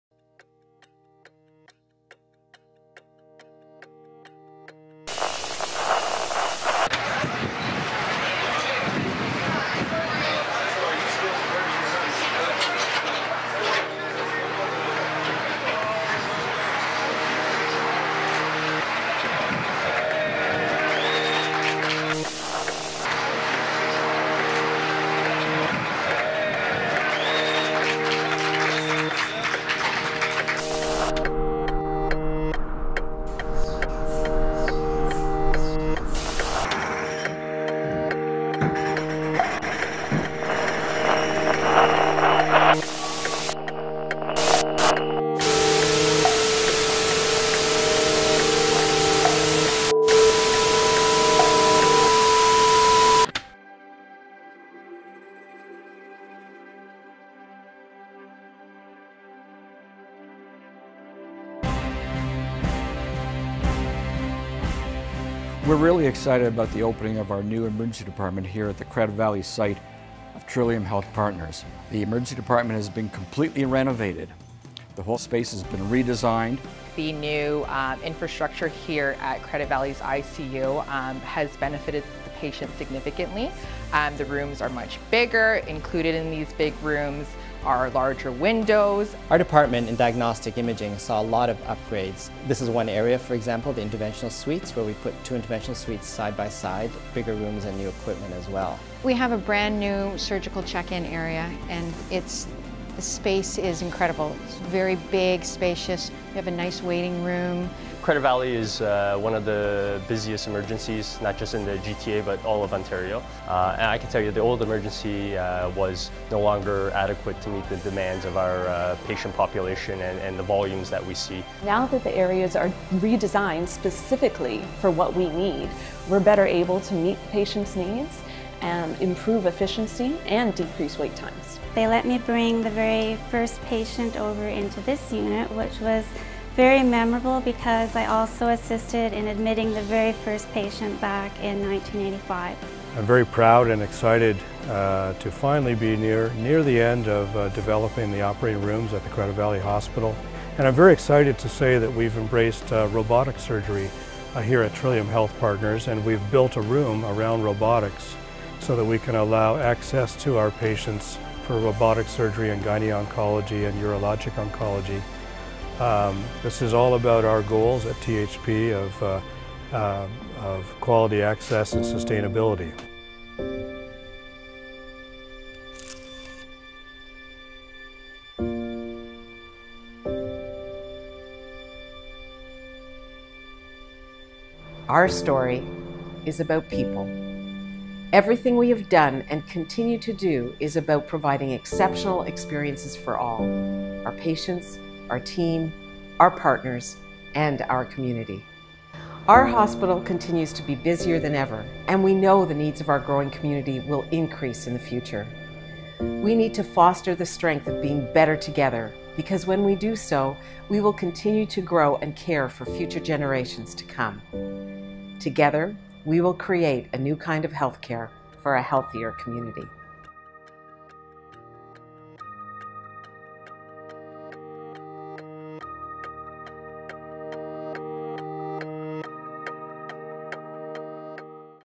Celebrating the Completion of THP's Credit Valley Hospital Redevelopment Project
THP is proud to celebrate the completion of our Credit Valley Hospital Redevelopment Project, which has improved patient experience, medical technology and created more space to care for the community.

Learn more about this most recent step in our mission to create a new kind of health care for a healthier community